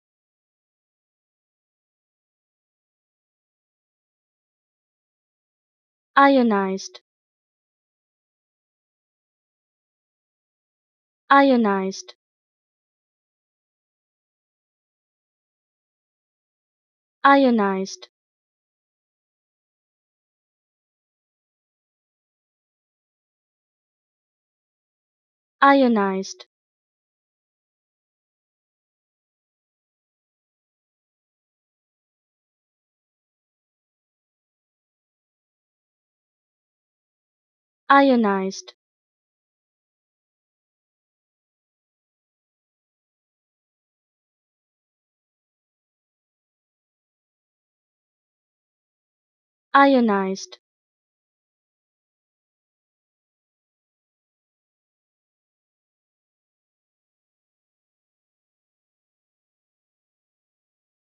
How To Say Ionised
Learn how to say Ionised with EmmaSaying free pronunciation tutorials.